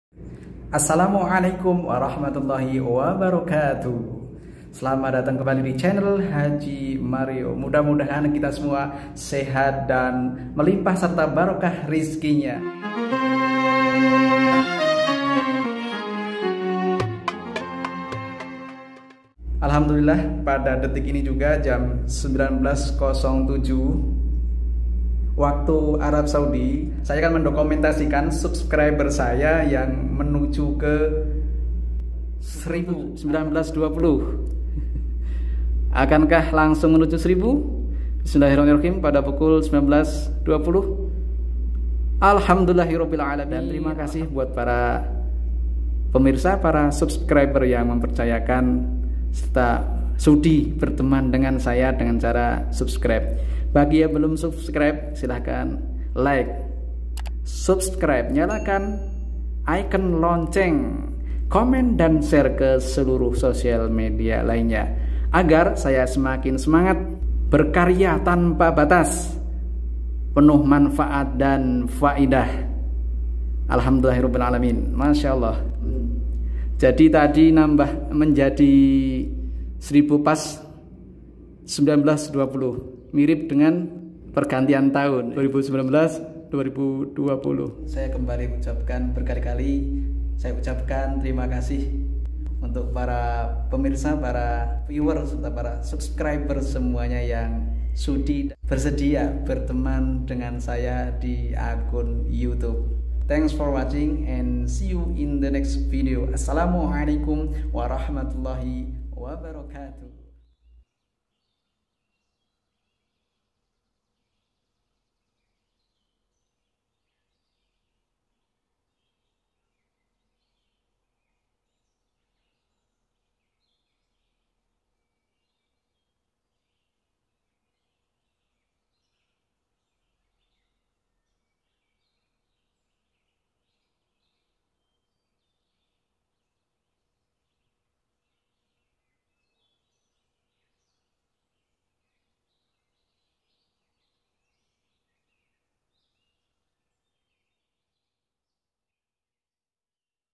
Cepat Capai 1000 Subscriber & 4000 jam,Terima Kasih Subscriber,Viewer & Semua Yang Telah Mensupport
Alhamdulillah ,1000 Subscriber dan jam tayang 4000 jam tonton adalah syarat mengajukan monetisasi,saya raih berkat kebaikan  Kalian Kawanku semua